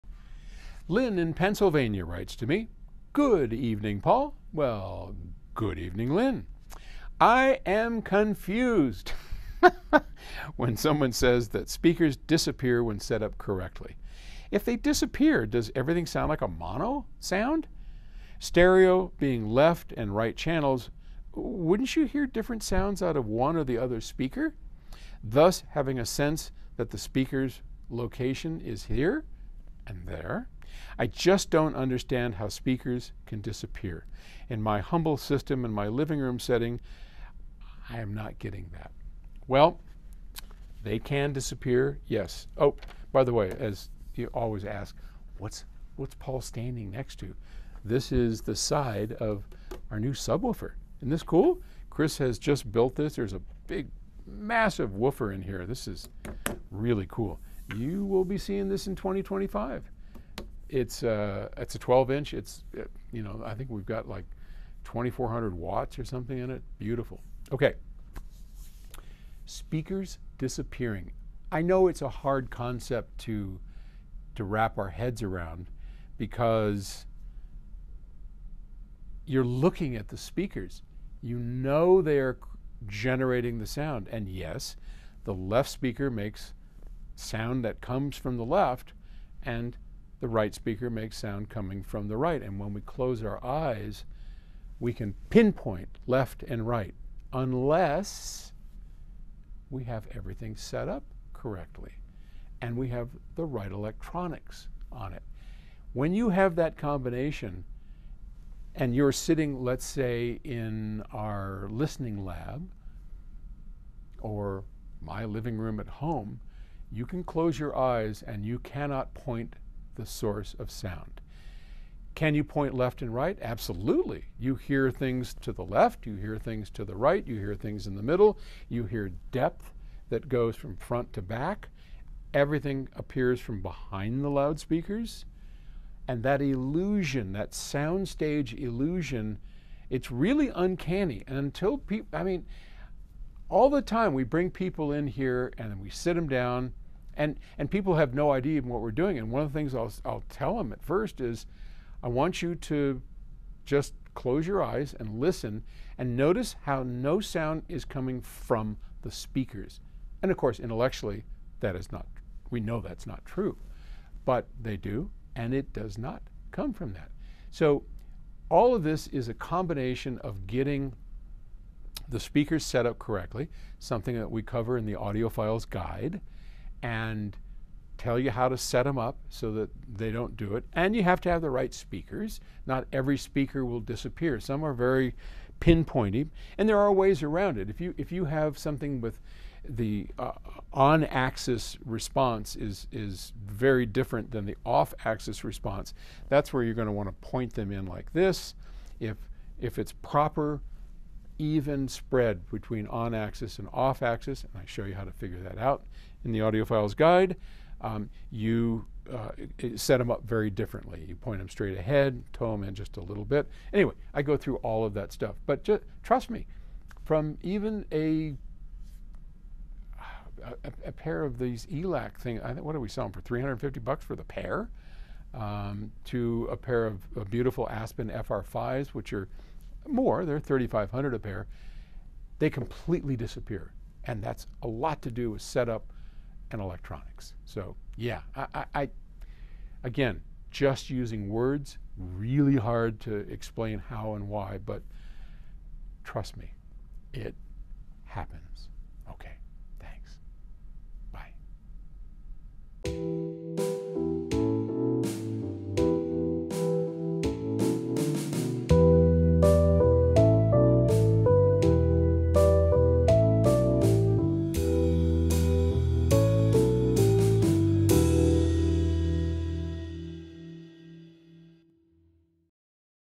How loudspeakers disappear
How is it possible big loudspeakers disappear in a high-end setup?